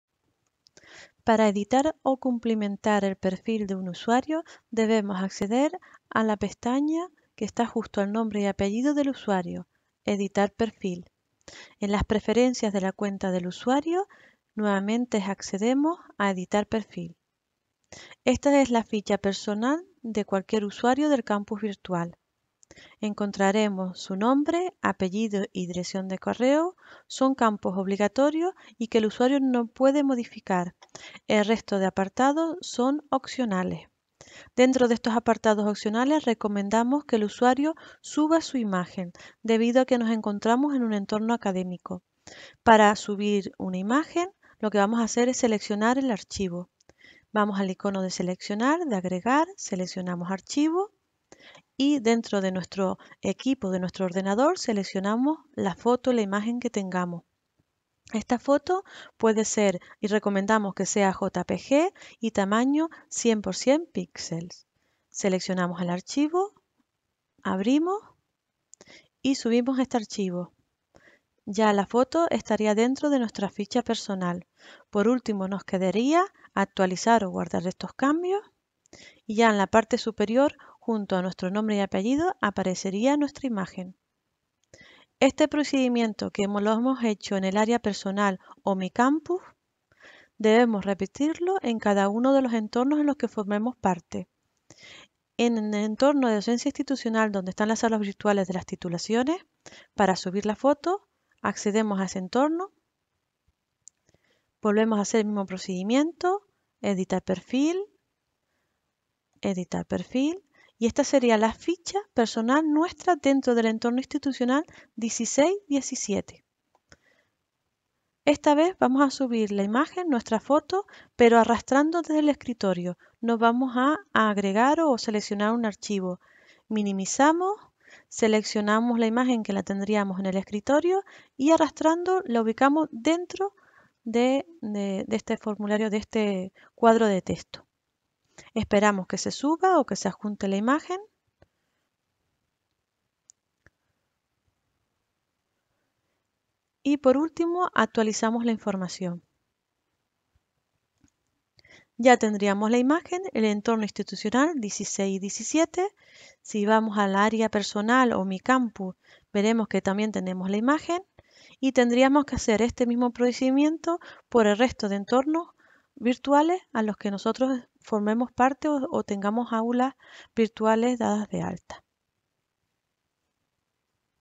Curso Competencia Digital - Campus Virtual ULL- Aulas virtuales: Edición del perfil del usuario
Curso Competencia Digital - En este vídeo se describe los pasos para editar y cumplimentar el perfil del usuario en el campus virtual. Este material pertenece al Curso Competencia Digital organizado por la UDV.